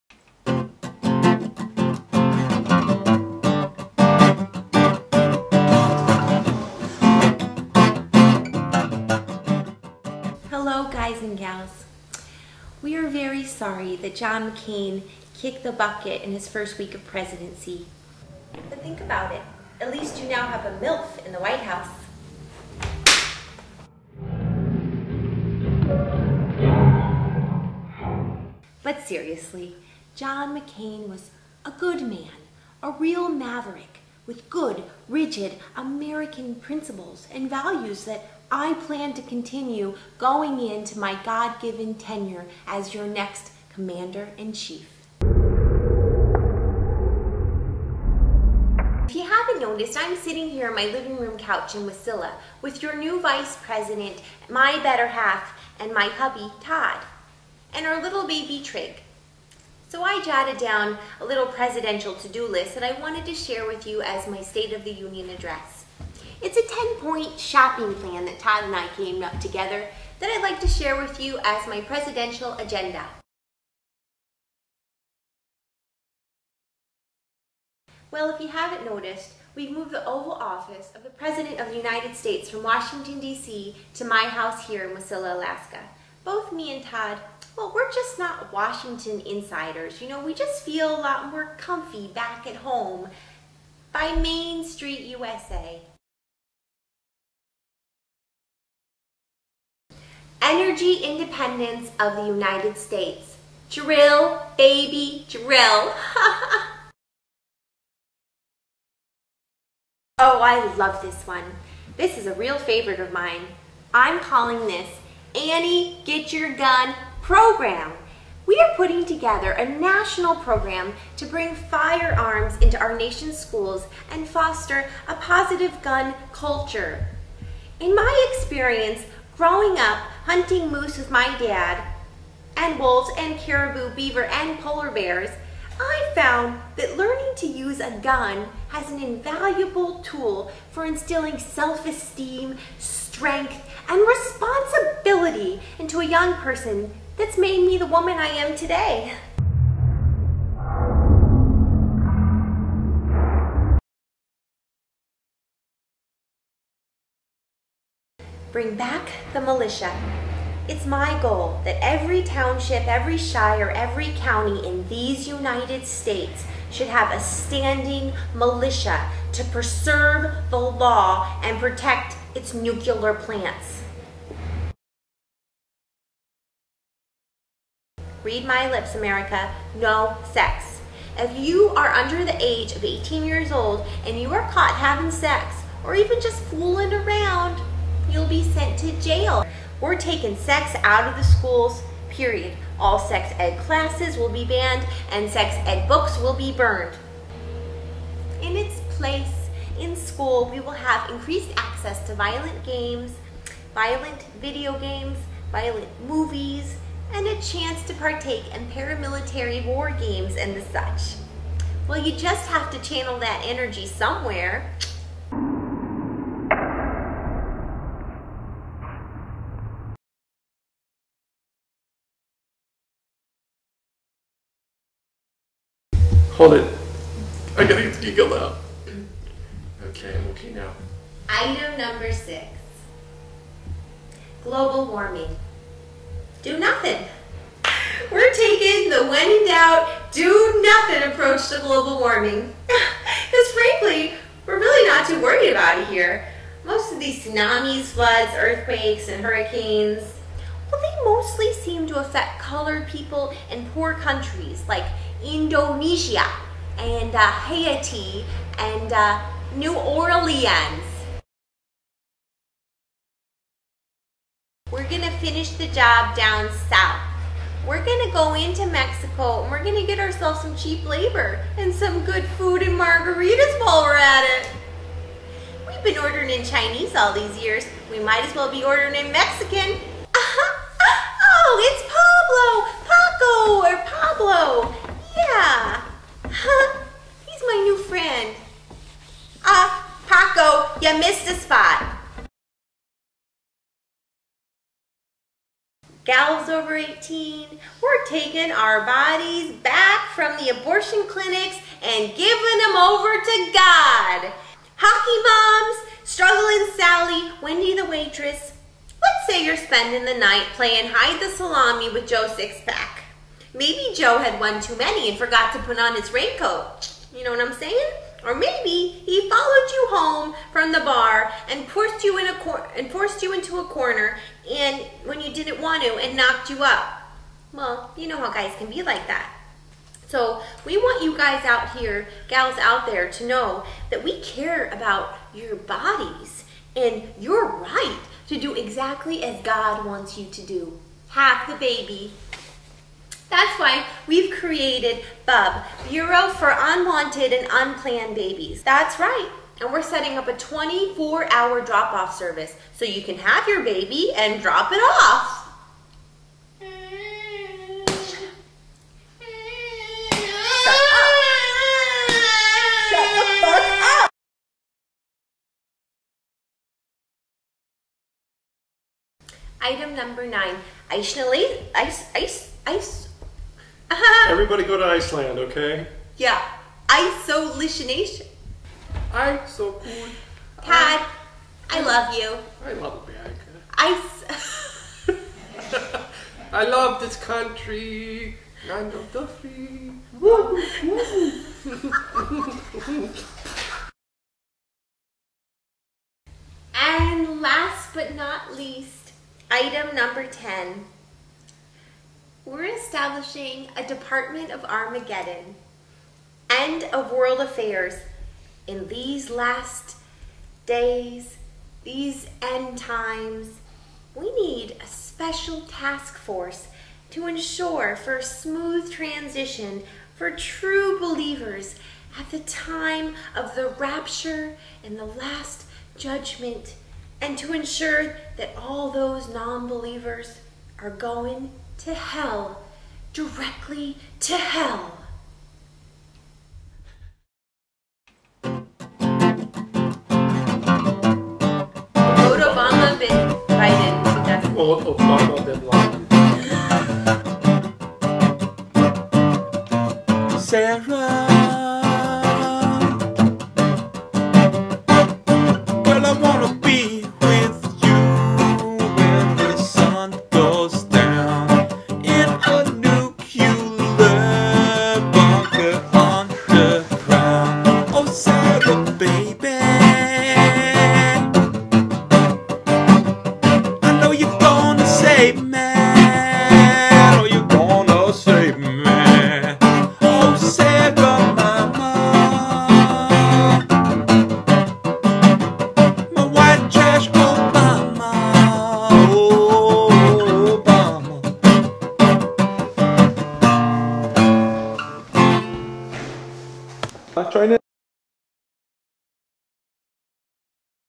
Sarah Palin's State of the Union
McCain died in his first week in office and Sarah's taken over. Here's her first address to the nation